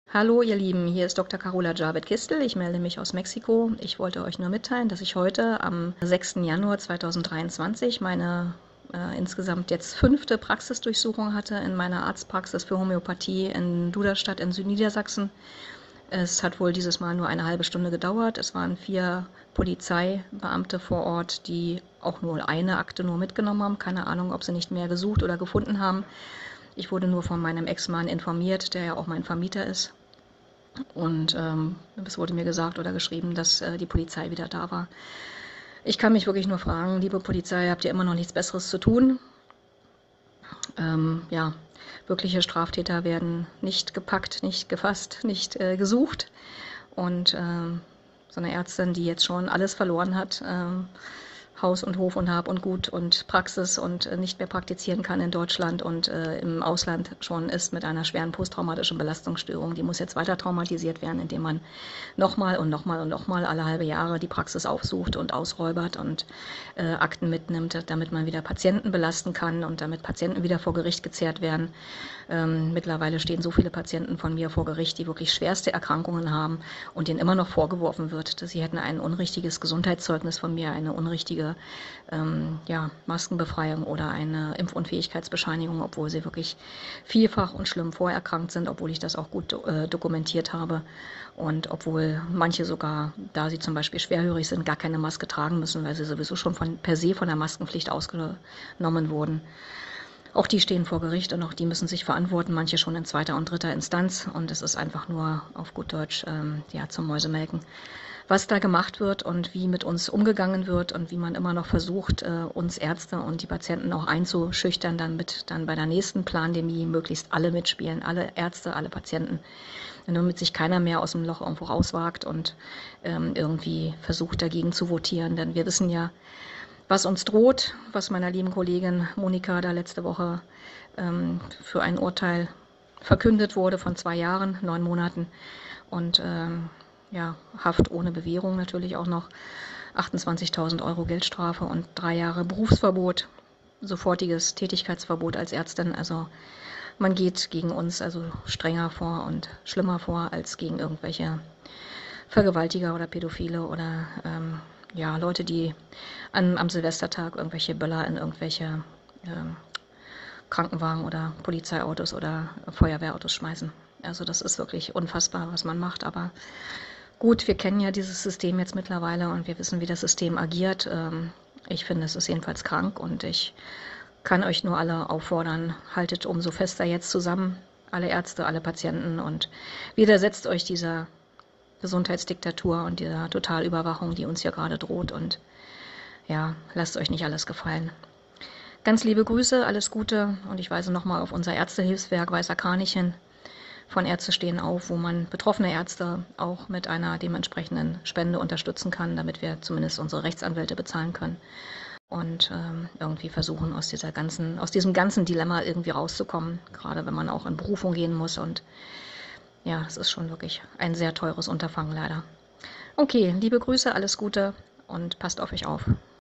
Fünfte Polizeirazzia in Arzt-Praxis Dr. med. Carola 👩🏼 Javid-Kistel - 06.01.2023 - Jagd auf Ärzte 💥
Die niedersächsische Ärztin befindet sich bereits seit fast einem Jahr in Mexiko.
5. Praxis-Durchsuchung - 6.1.23

Am 02.01.23 wurde Dr. Monika Jiang aus Weinheim ohne Bewährung zu 2 Jahren u. 9. Monaten Freiheitsstrafe, sofortigem Berufsverbot und 28.000 EUR Geldstrafe verurteilt. (Urteil noch nicht rechtskräftig da Staatsanwalt u. Beklagte sind in Revision gegangen sind)